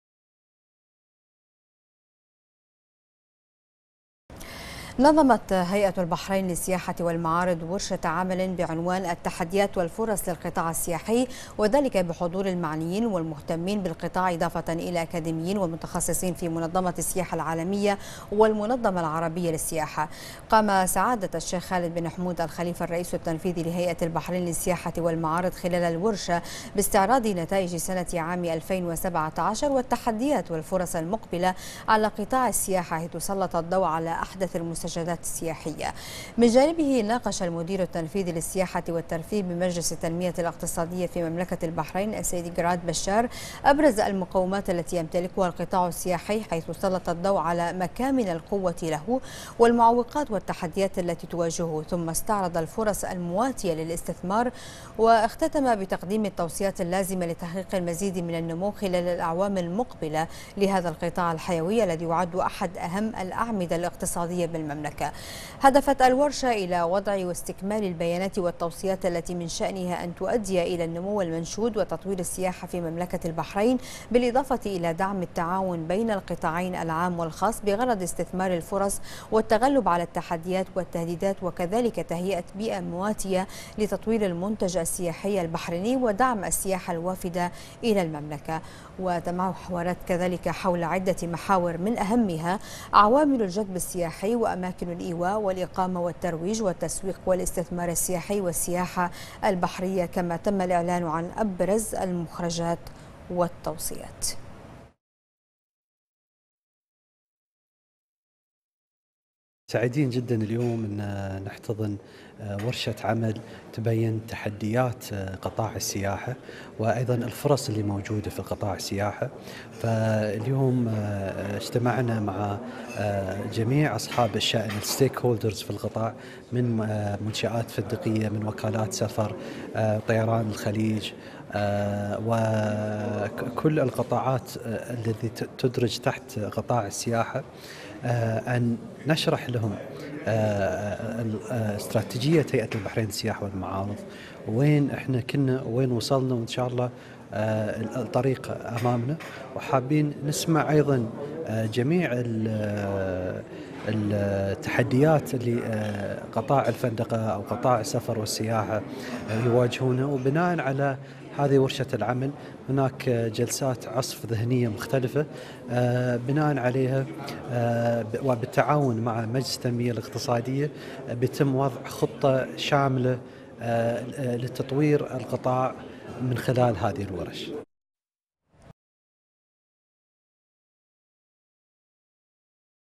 البحرين : هيئة البحرين للسياحة والمعارض تنظم ورشة عمل حول التحديات والفرص
هيئة البحرين للسياحة والمعارض تنظم ورشة عمل حول التحديات والفرص